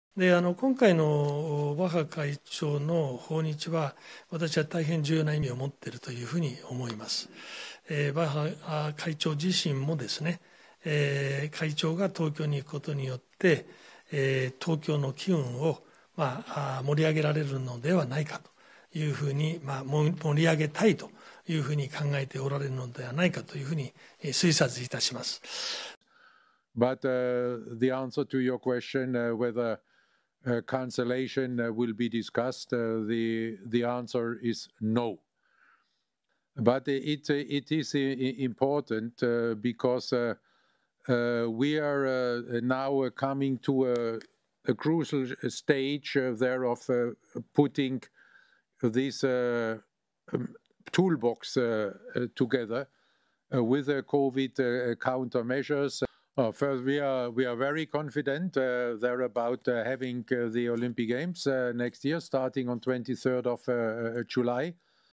Tokyo 2020 CEO hopes Bach's Japan trip will reignite Olympic excitement
Tokyo 2020 CEO Toshiro Muto said Thursday that International Olympic Committee (IOC) President Thomas Bach's visit to Japan next week could help generate excitement towards the games.

Cancellation of the Tokyo Olympic Games will not be on the agenda when Thomas Bach travels to the Japanese capital next week.